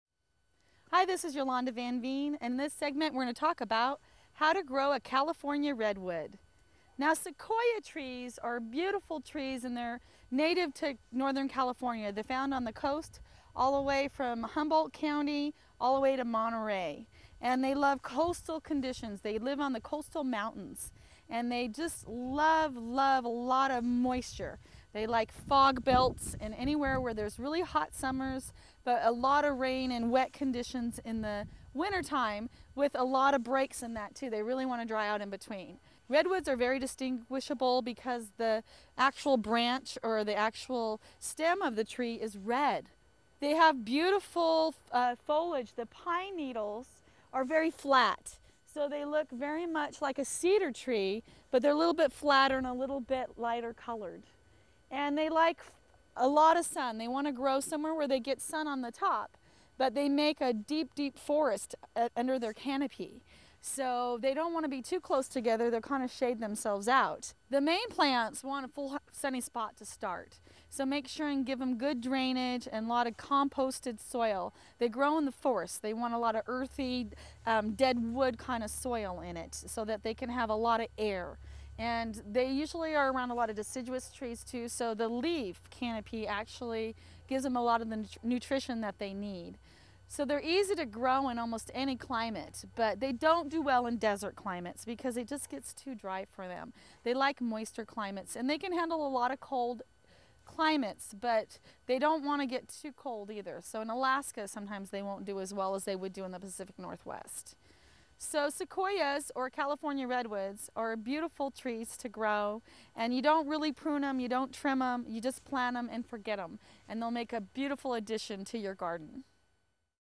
Tree & Plant Care : How to Grow California Redwoods
To grow California redwoods, or sequoias, use woody, composted soil, provide lots of water, good drainage and room to grow so the tree can get sun. Avoid trying to plant California redwoods in desert climates or very cold areas considering information from a sustainable gardener in this free video on gardening.